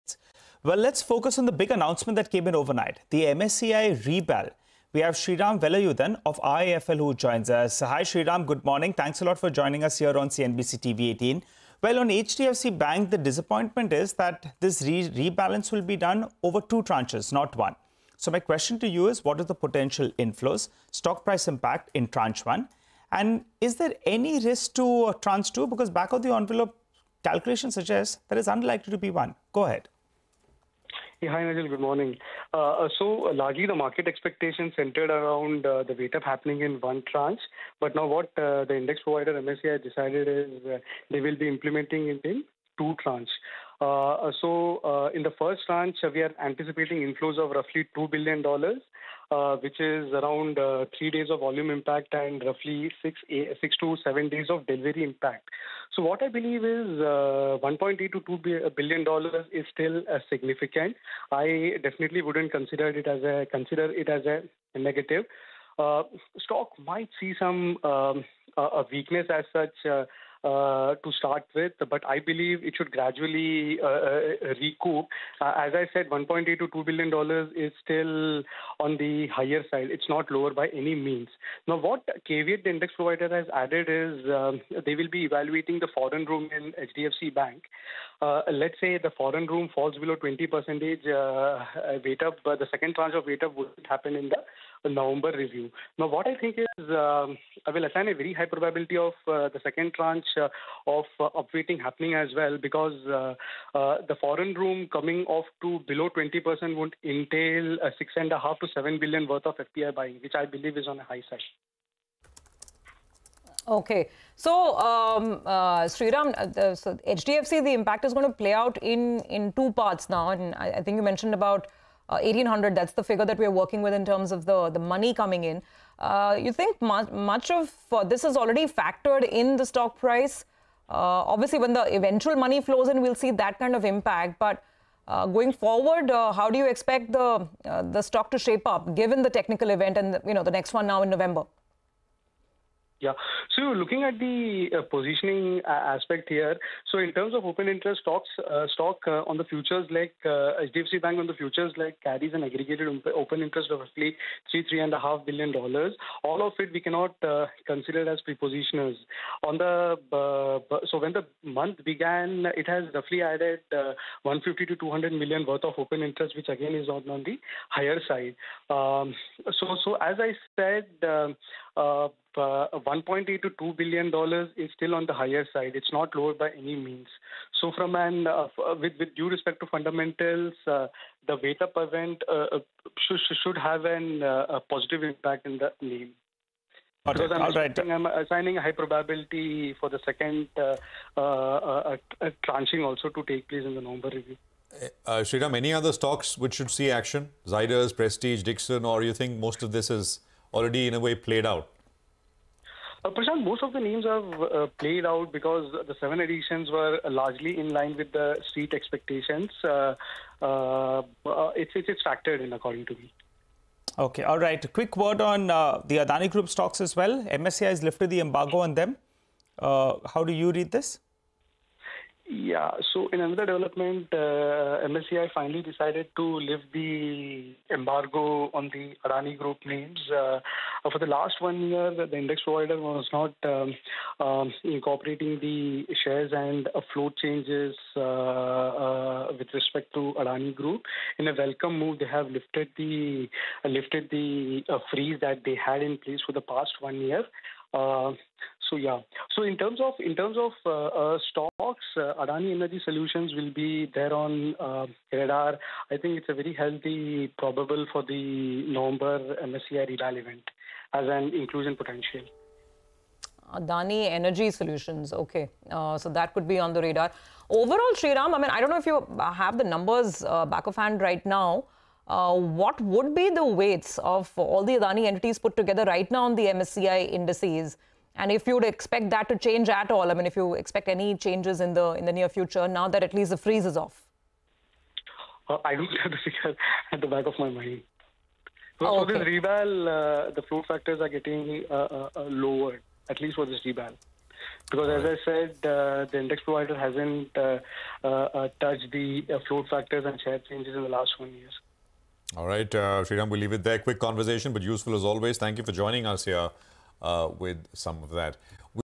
Most MSCI Inclusions Are In-Line With Street Expectations: IIFL Alternative Research | CNBC TV18
On CNBC-TV18 | Expect close to $1.8-2 bn inflows for HDFC Bank, says Sriram Velayudhan of IIFL Alt Research. He tells Prashant Nair, Surabhi Upadhyay and Nigel D'Souza that most MSCI inclusions are in-line with the street expectations

n18oc_business